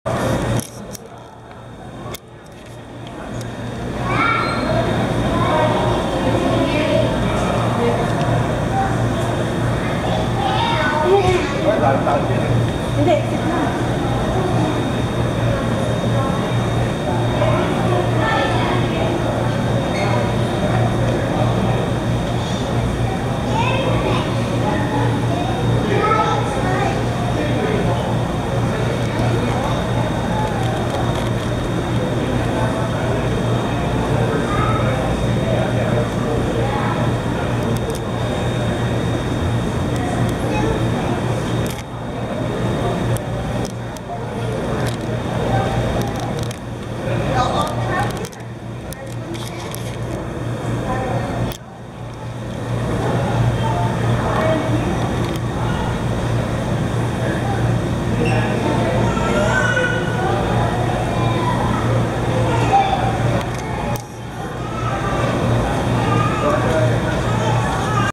Grunt Sculpin @ Vancouver Aquarium
Watch Closely: it uses its side (pectoral) fins for crawling instead of swimming.